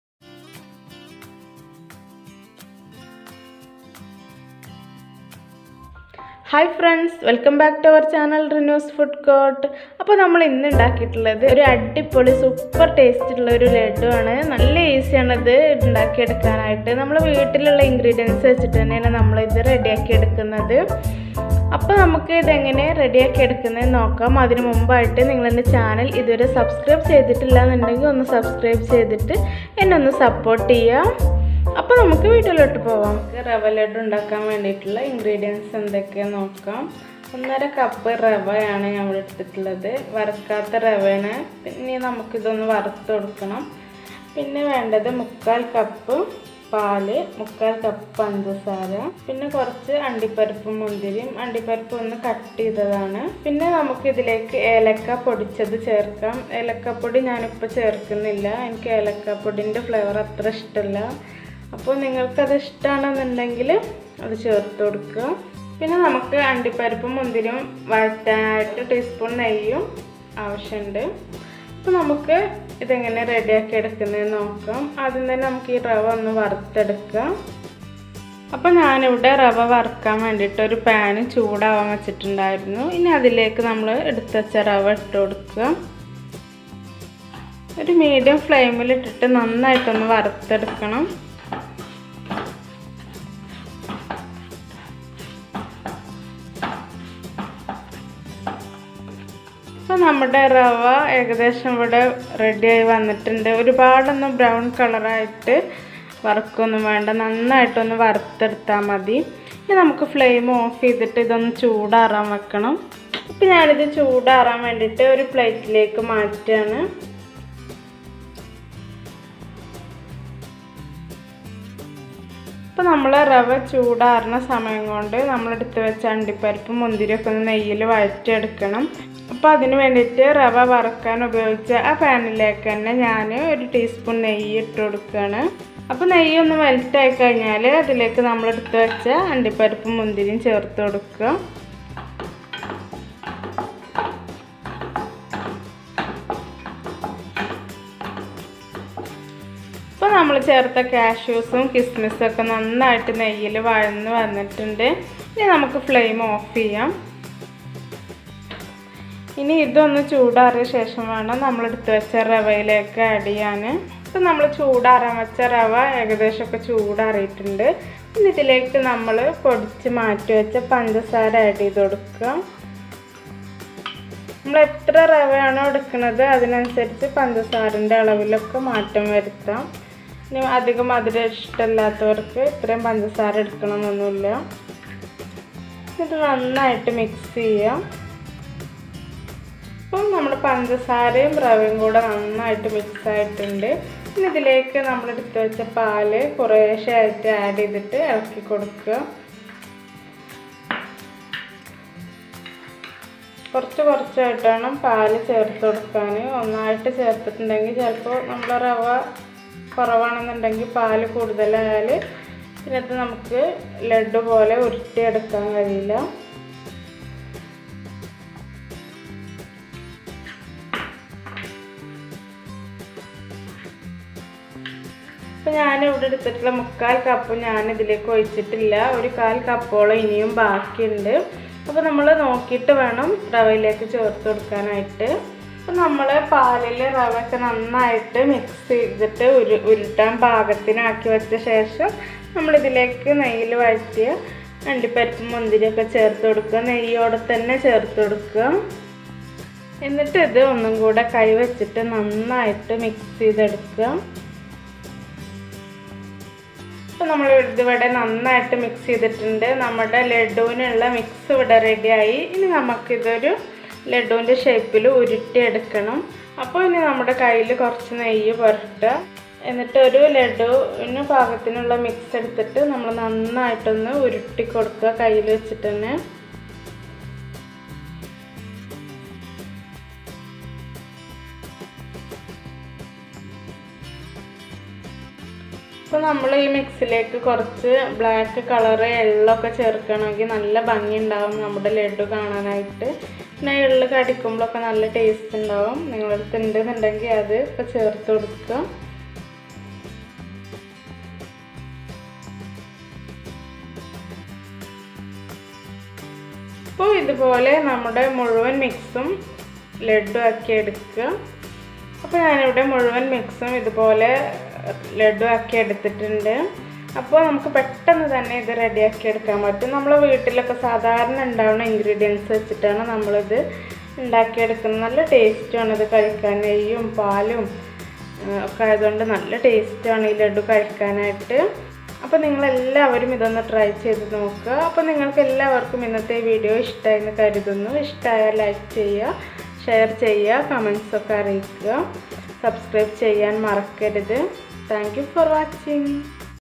Rava Laddu/Laddu recipe/Easy to make/Super taste/Malayalam
Ingredients

Rava
Sugar
Milk
Ghee
Cashew
 Kismis